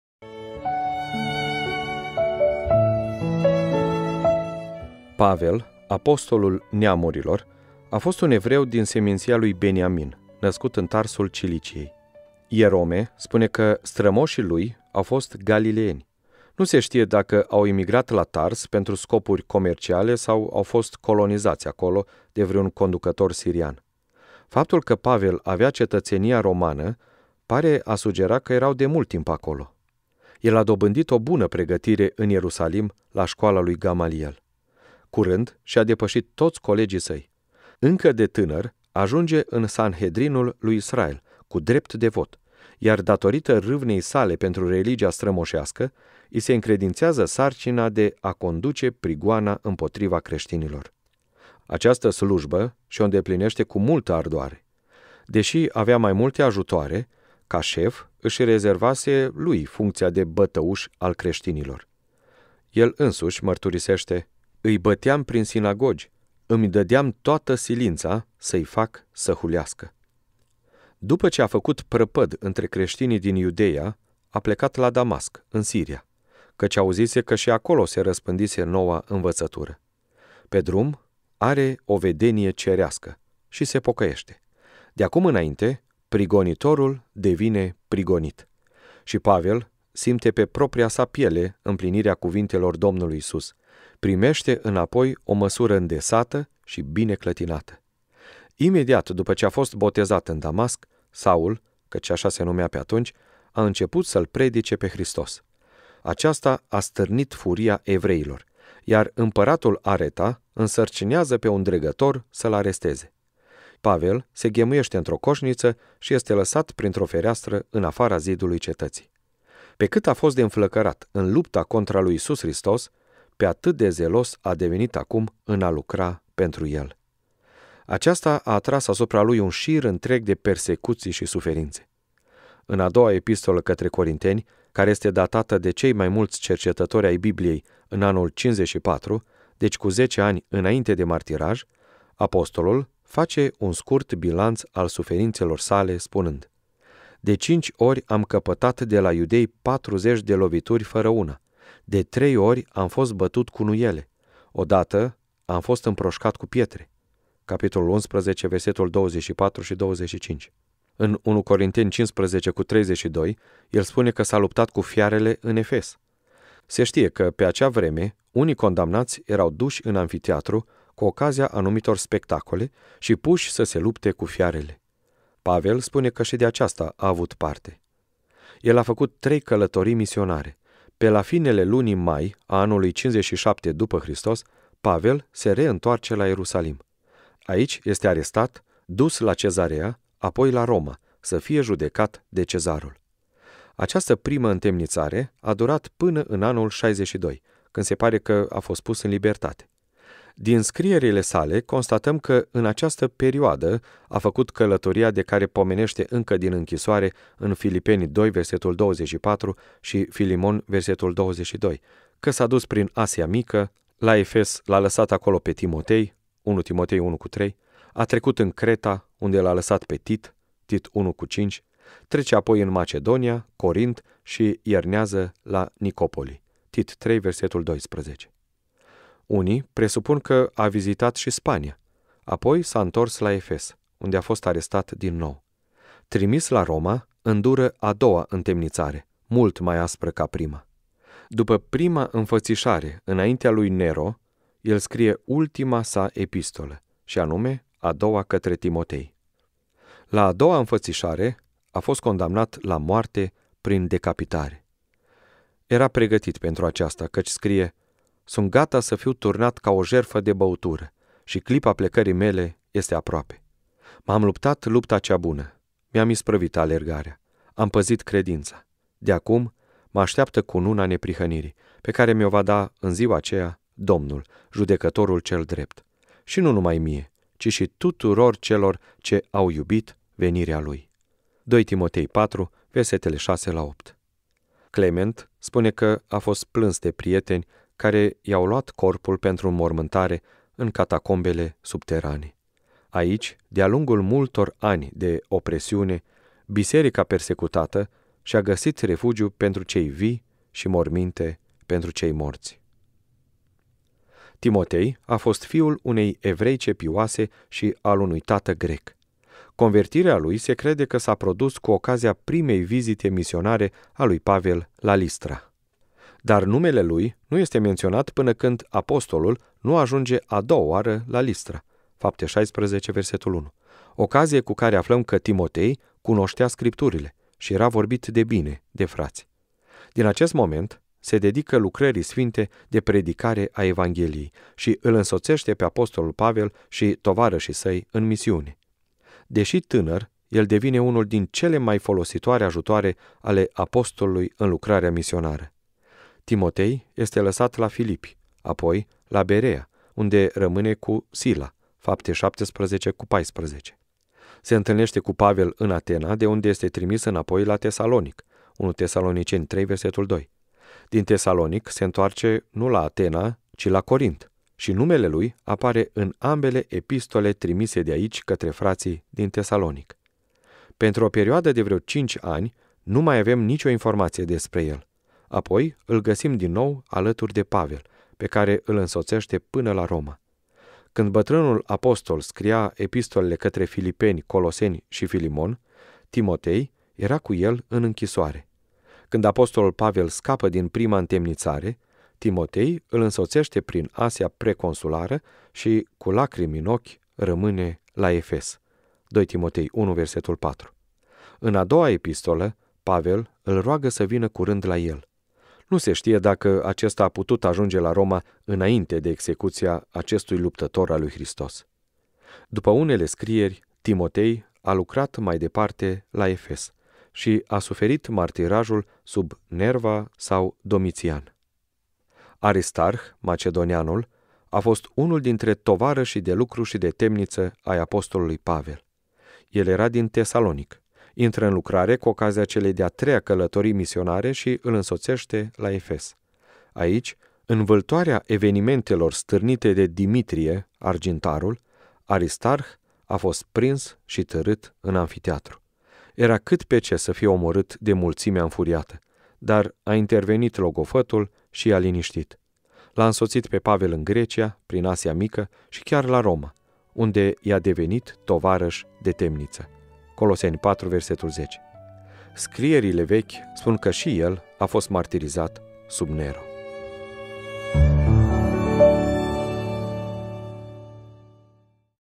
(4) Graiul martirilor - Pavel, Timotei, Aristarh
Mărturii ale primilor creştini care din dragoste pentru Hristos nu şi-au iubit viaţa până la moarte de martir.